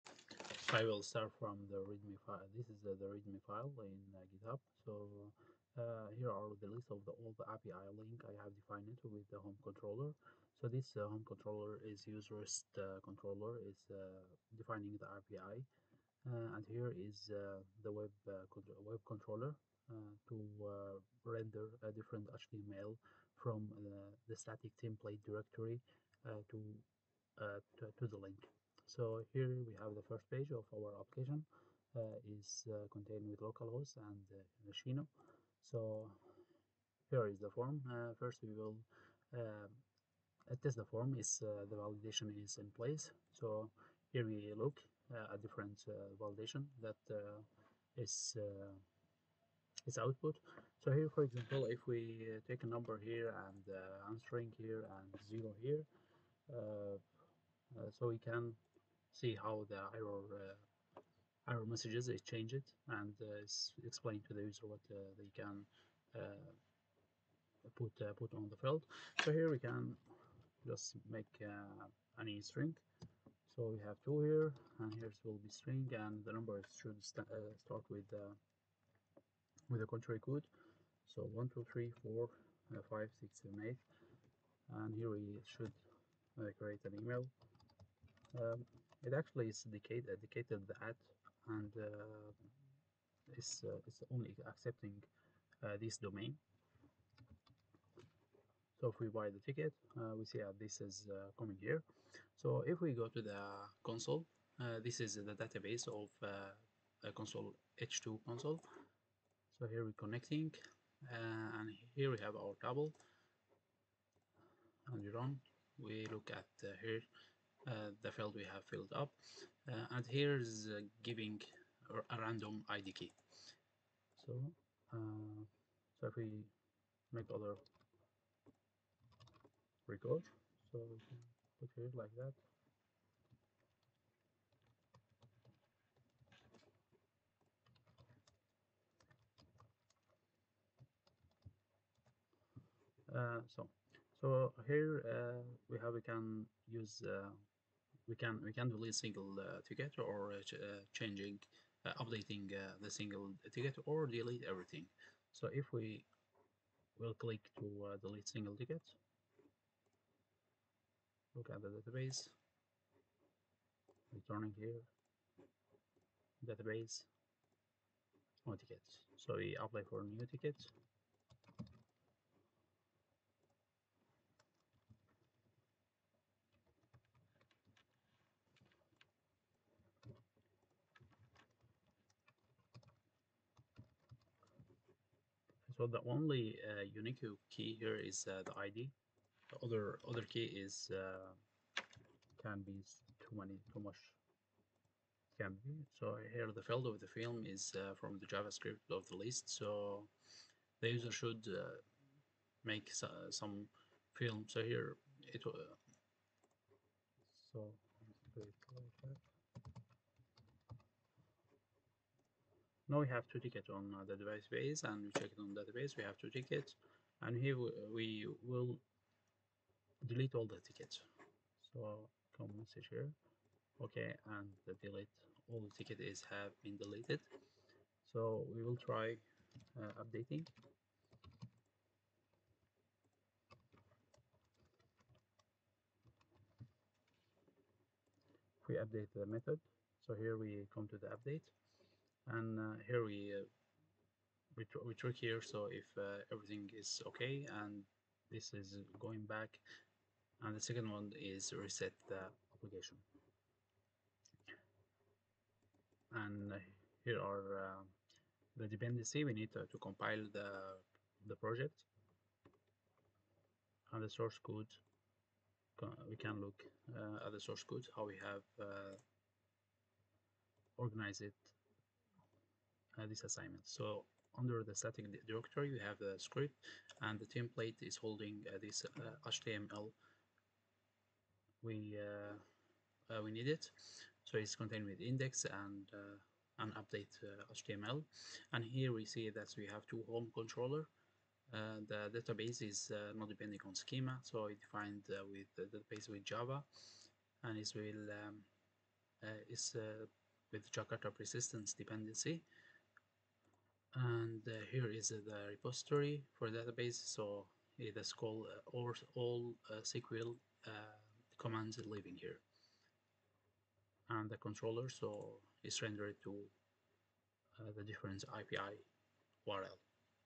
Creating a form with Spring boot and style with Bootstrap
Capture the code from GitHub repository.

This project is ticket reservation form, created with H2-database with CRUD feature.

Basic

Testing ticket reservation forms